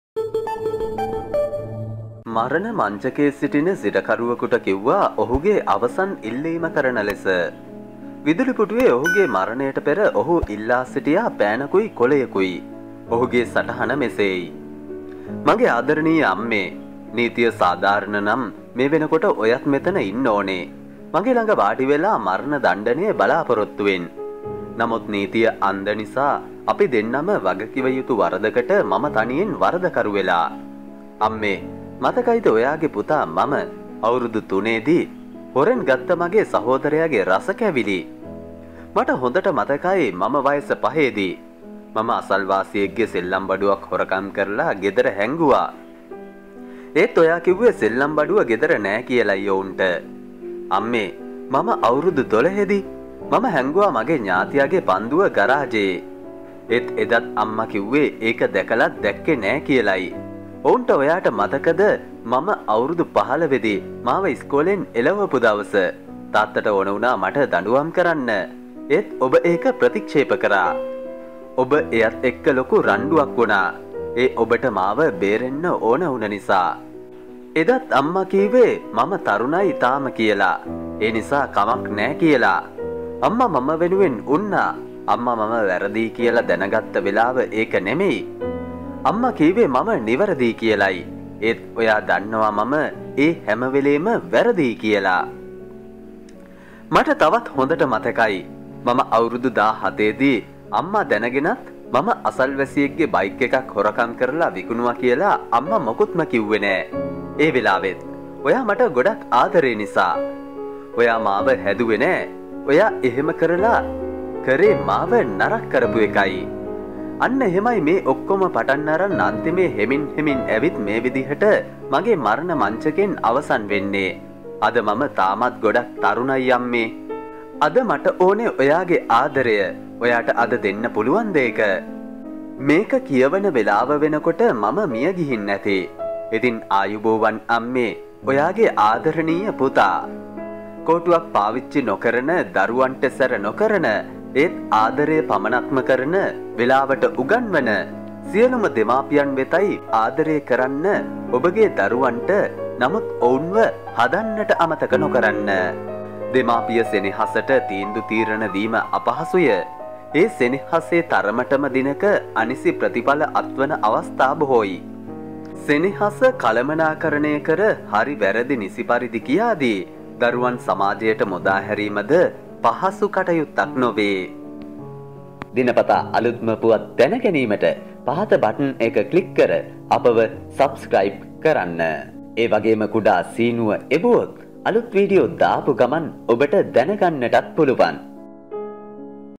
විදුලි පිඋටුව උඩ ඉඳගෙන ඔහු ලෝකයටම හැදෙන්න ලියපු අන්තිම වචන පෙළ - අනිවා බලන්න
විදුලි පුටුවට ගෙනගිය ඔහු අවසන් කැමැත්තට ඉල්ලුවේ පෑනකුයි කොලයකුයි ඔහු එහි ලියපු දේ දැක්ක ජේලර් වරුන්ගේ ඇස් උඩ ගියා